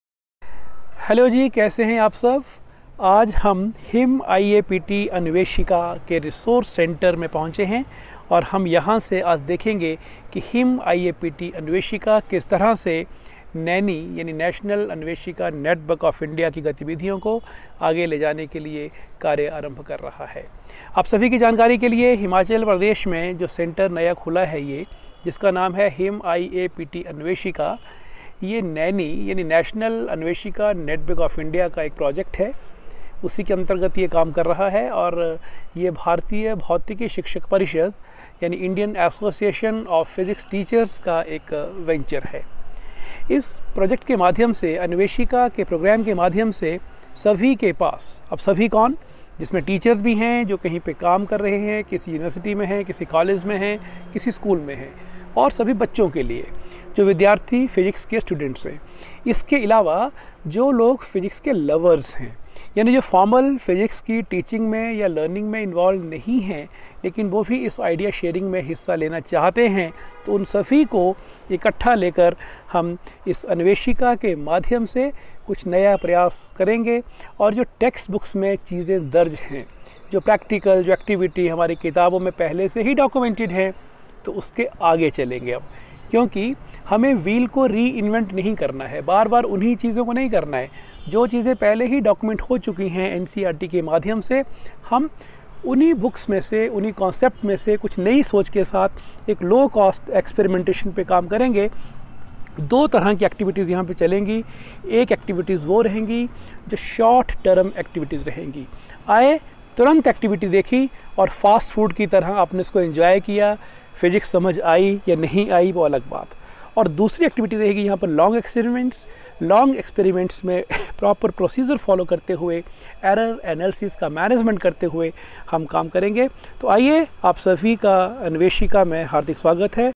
HIM - IAPT Anveshika resource centre
this video is about the resource centre of HIM - IAPT Anveshika of National Anveshika Network of India.